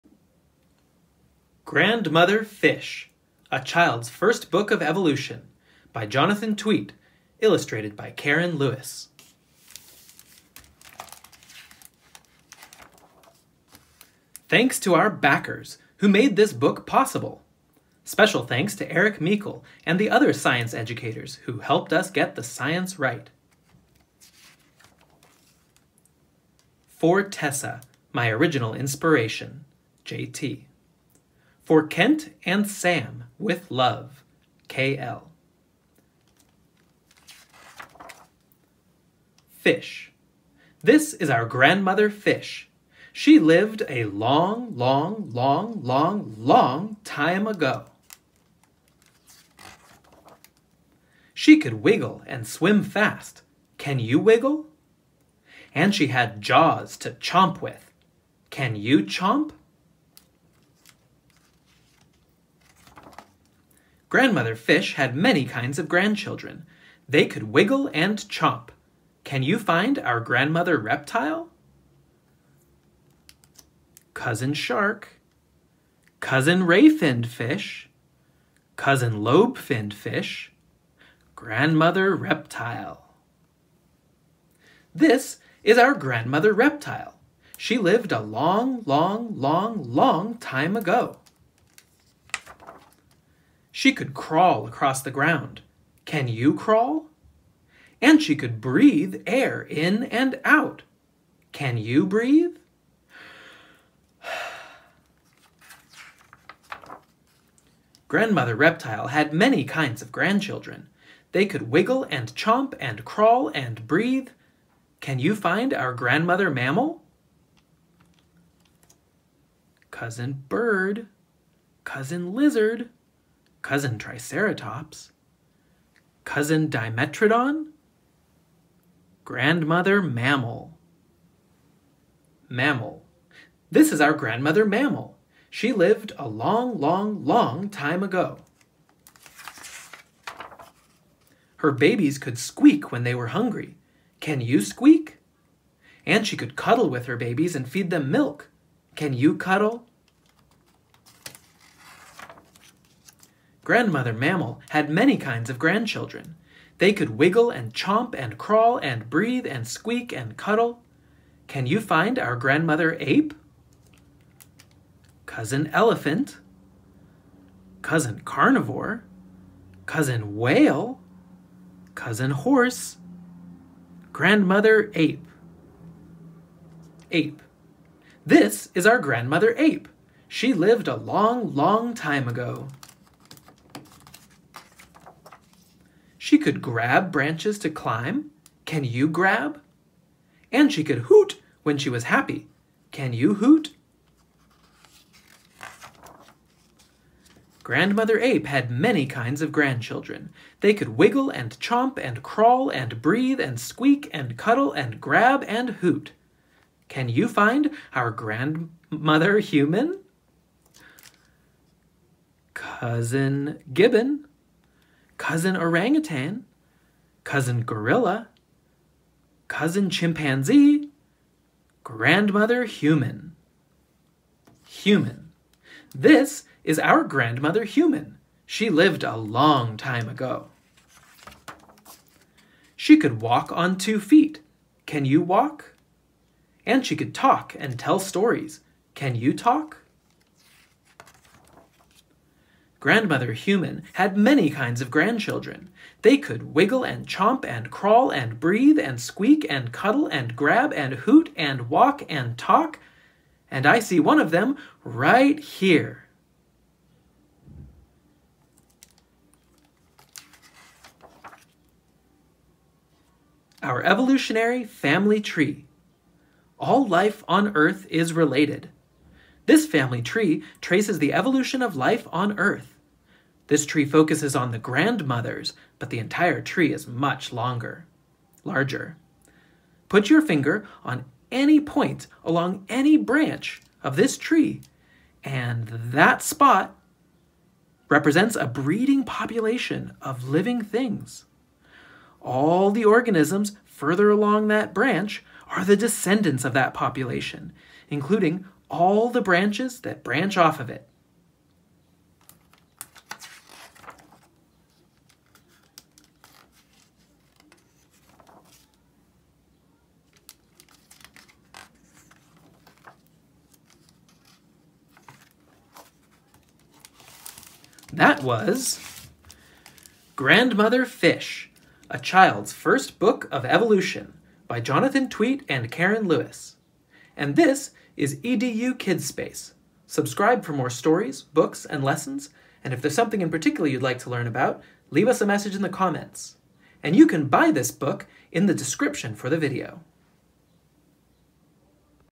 Grandmother Fish: A Child's First Book of Evolution, by Jonathan Tweet and Karen Lewis
"Told in an engaging call and response text where a child can wiggle like a fish or hoot like an ape and brought to life by vibrant artwork, Grandmother Fish takes children and adults through the history of life on our planet and explains how we are all connected.

*I am a Montessori Elementary teacher and educational content-creator, who started a youtube channel while on pandemic lockdown in New York City.